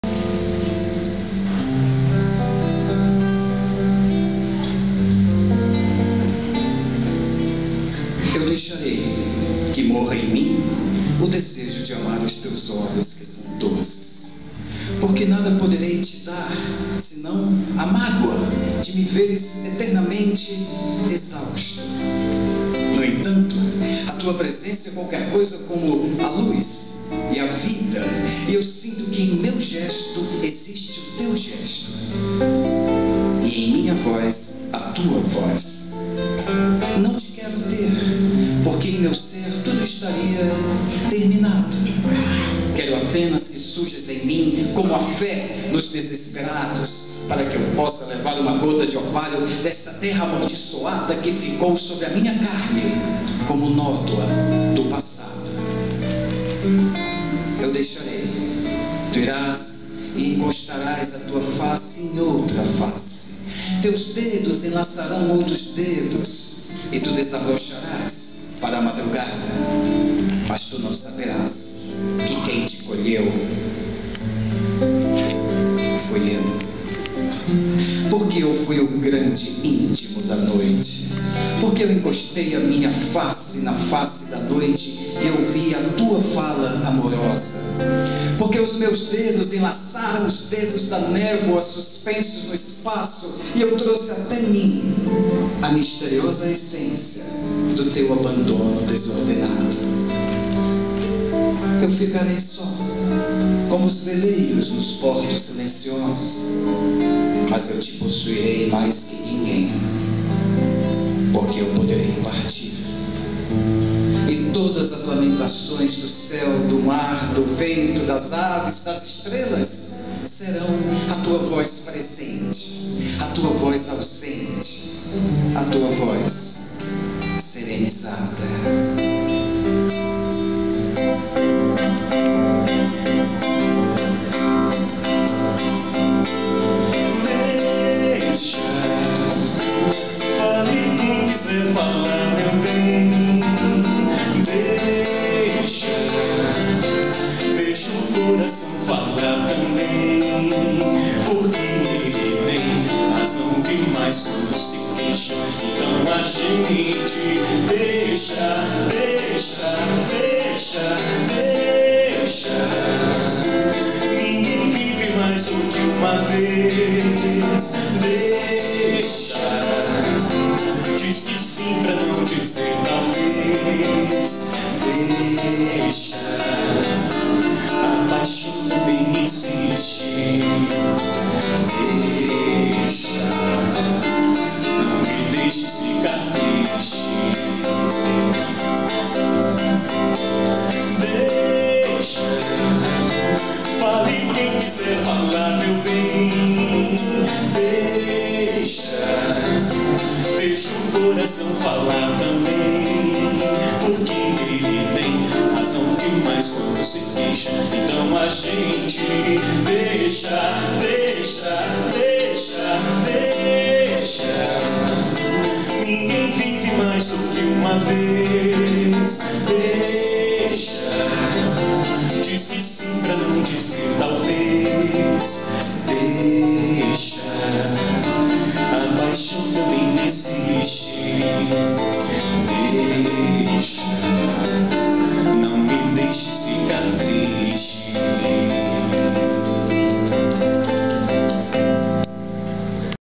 Vinicius Show de Moraes - Ricardo Kelmer e Felipe Breier
Ricardo Kelmer recita poesias e Felipe Breier  canta os clássicos musicais de Vinícius de Morais, homenagem aos 100 anos de nascimento do poeta em 2013

Encontro para a Nova Consciência 2013 - em Campina Grande - Paraíba (09 de fevereiro de 2013)